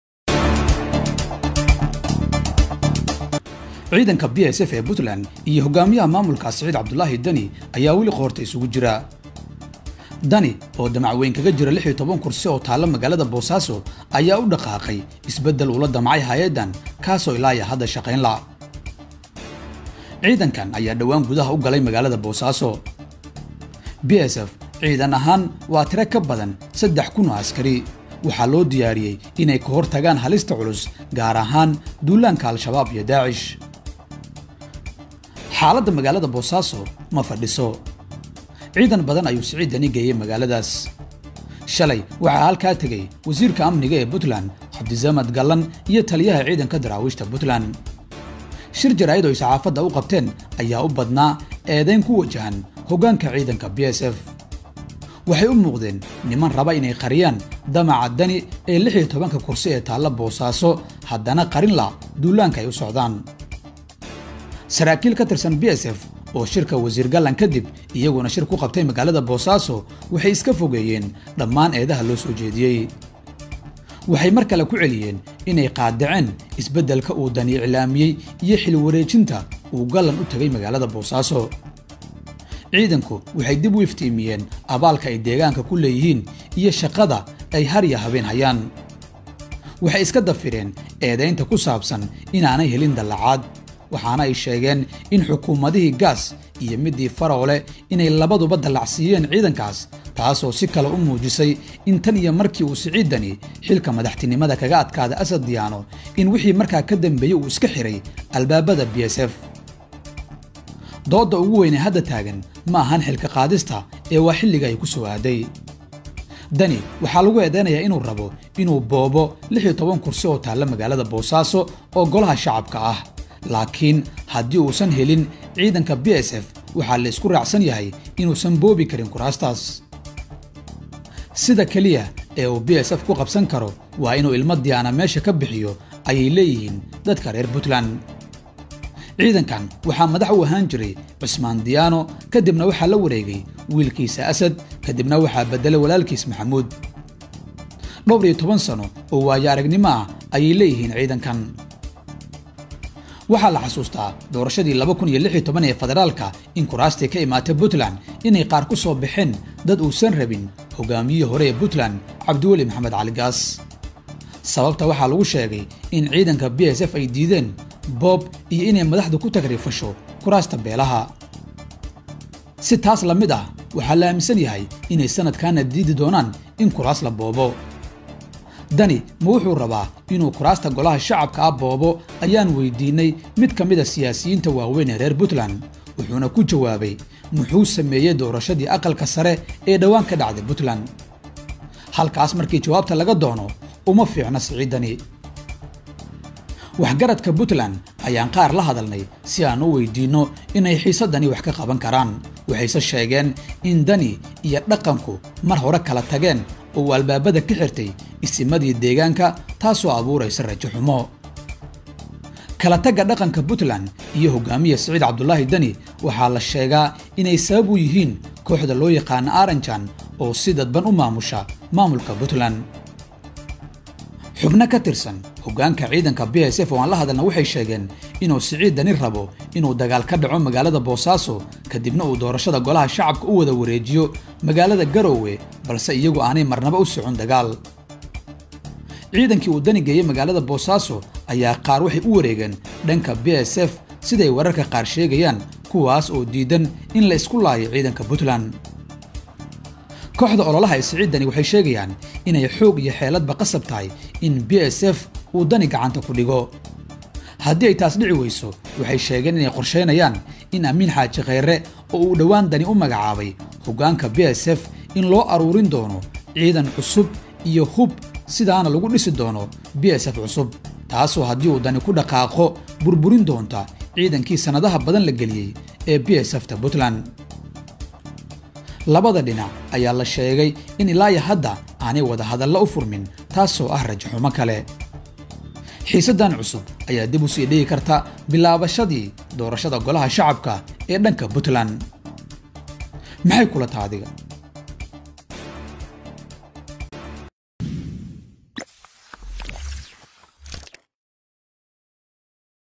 Xaalka Bosaso & Xiisadda Ka Dhex Taagan Deni Vs PSF......"Abdullah Osman".....
Siciid Deni ma xoogi karaa 16-ka kursi ee Bosaso taala?..